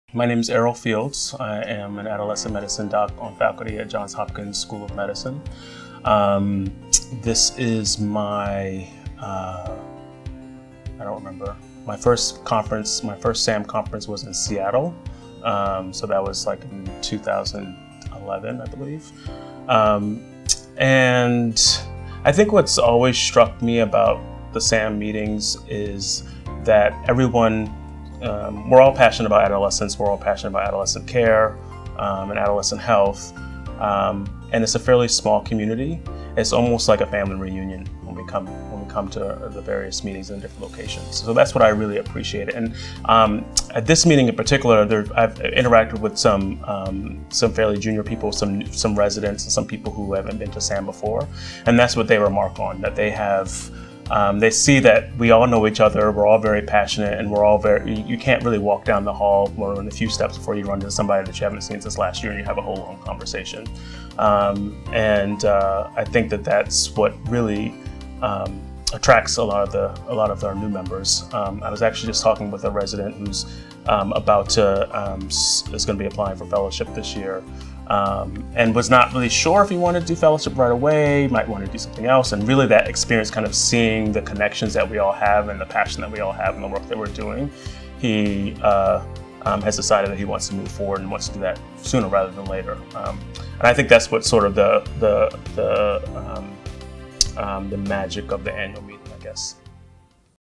Errol L. Fields, MD, MPH, PhD - Johns Hopkins
Dr. Errol L. Fields discusses why coming to SAHM is like a big family reunion.